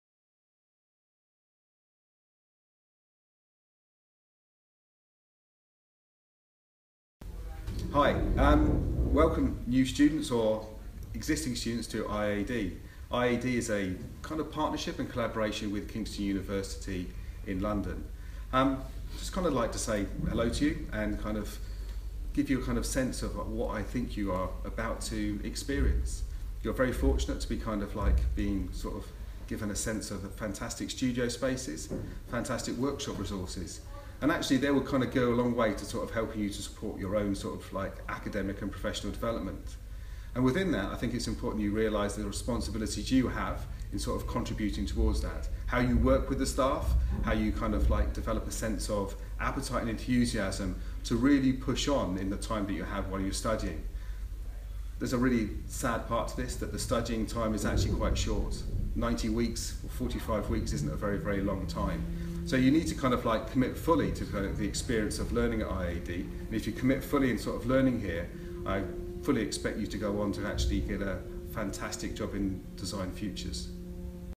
Welcome Message by Simon Maidment, Former Head of the Design School, Kingston University, London
Mr. Simon Maidment, former Head of the Design School at Kingston University, UK, shared a welcome message for the Foundation Year students as they begin their degree programmes at the Indian Institute of Art and Design.